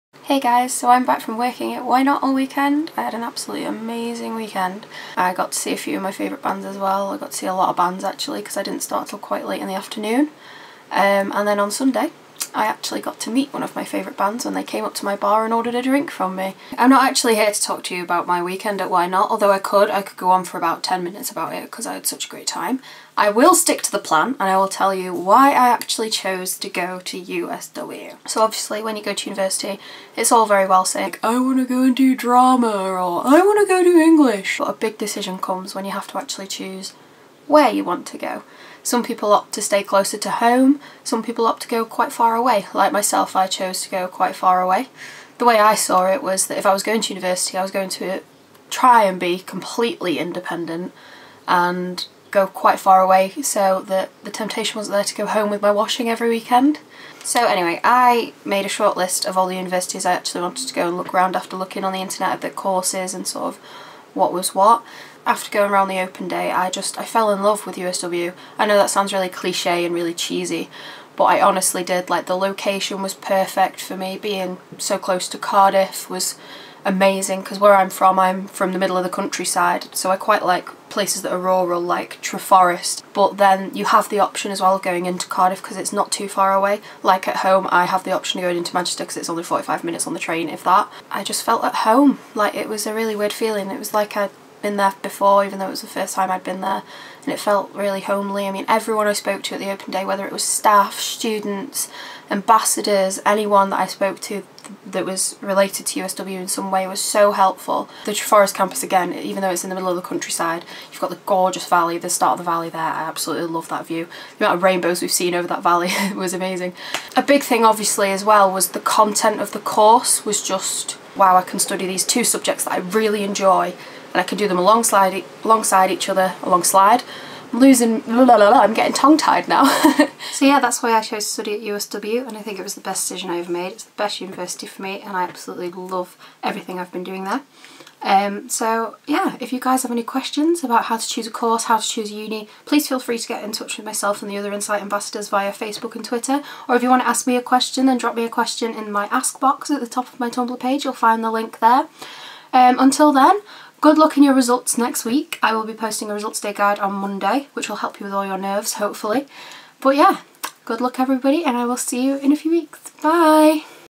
Why I chose my University - Student Vlog
Hi guys, I'm fresh back from working at Y Not Festival and I wanted to go through why I chose to study at the University of South Wales. Hopefully it will help with your decision. Good luck with results day!

Any questions or comments let me know!

Rhian